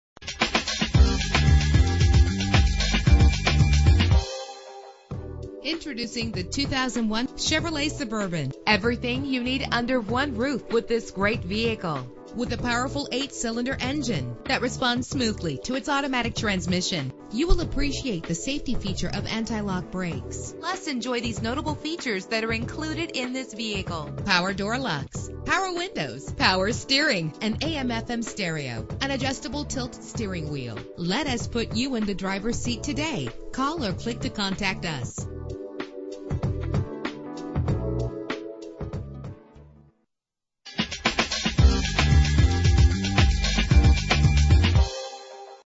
2001 Chevrolet Suburban - LAGRANGE GA
Year: 2001
Make: Chevrolet
Model: Suburban
Trim: 1500
Engine: 5.3 liter 8 cylinder 16 valve
Transmission: 4-Speed Automatic
Color: Gold
Mileage: 168756
Address:
709 NEW FRANKLIN RD